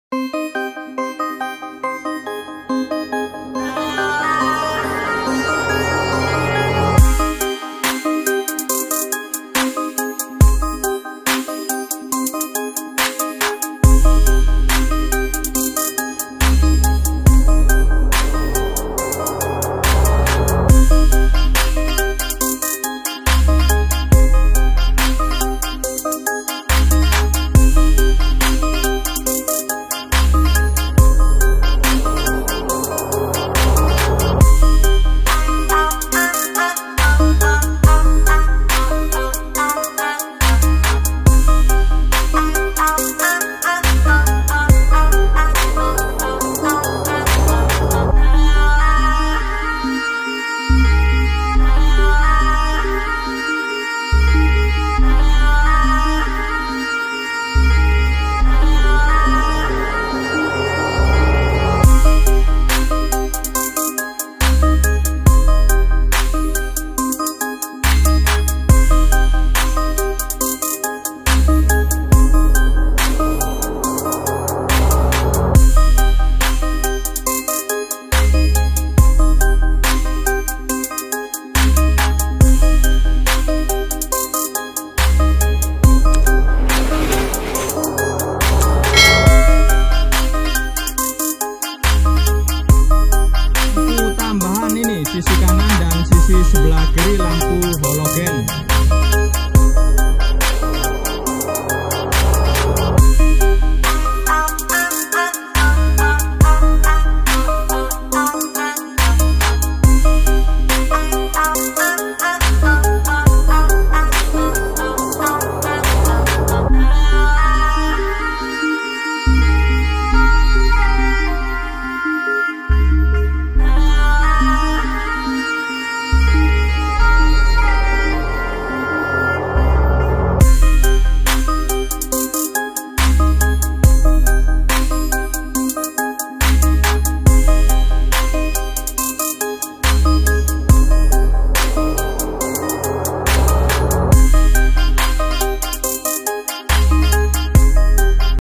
INTERIOR MBOIS L300 PICK UP
assalamualaikum warahmatullahi wabarakathu..

dikesempatan yang sangat istimewa ini saya berkesempatan untuk meriview mobil pick up Mitsubishi L300 yang sangat elegan, dengan sentuhan-sentuhan modifikasi molai bagian interior dan exterior.

bila ada kekurangan dalam menyajikan Vidio,,mohon untuk komen kritik & sarannya dikolom komentar ya, supaya saya terus berbenah dalam menyajikan Vidio yang berkualitas..

jangan lupa saling support ya..